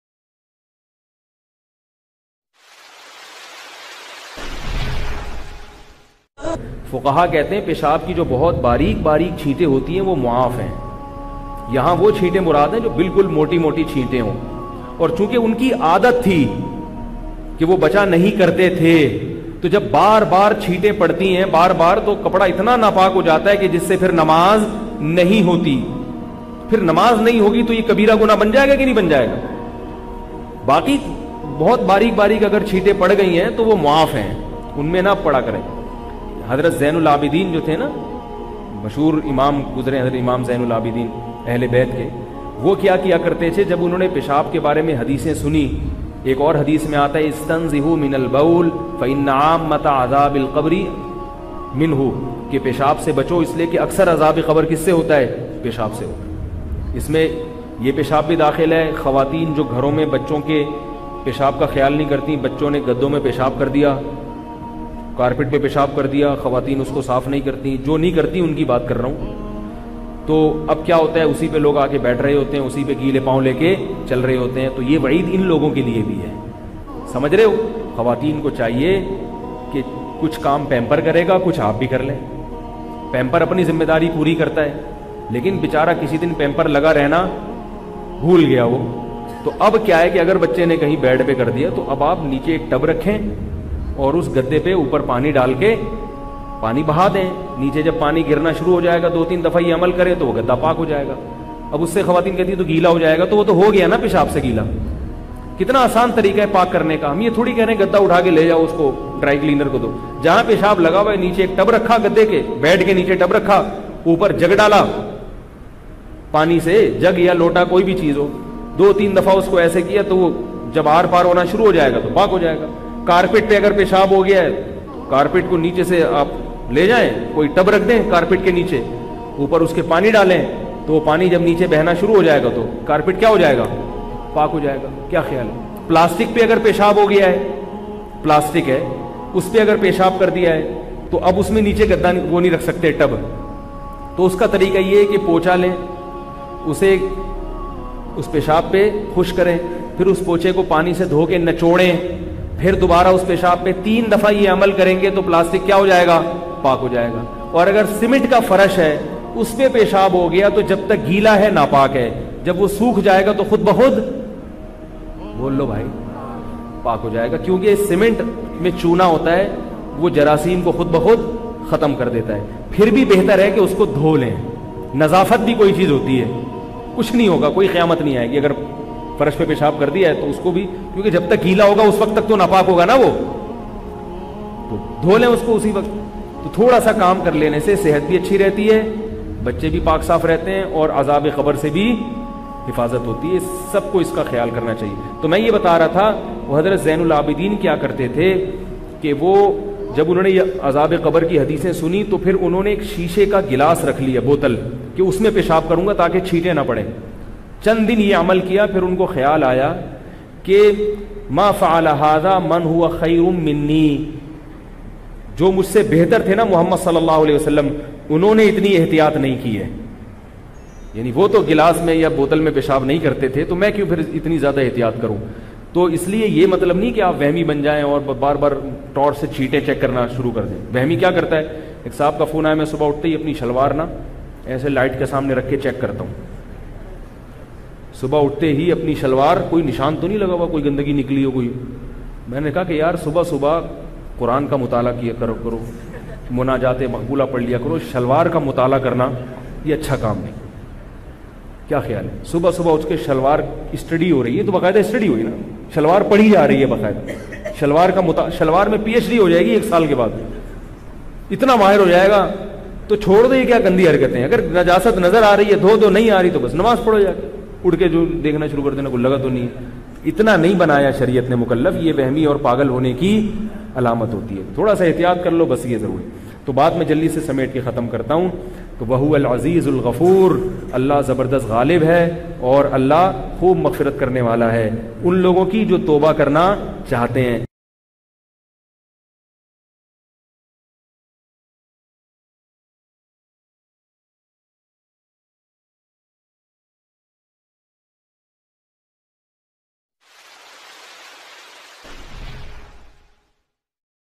[An Special message for All muslims] ek esa gunah | Popular Media.
[An Special message for All muslims] ek esa gunah jo hamare liye qabar ka azab ban sakta ha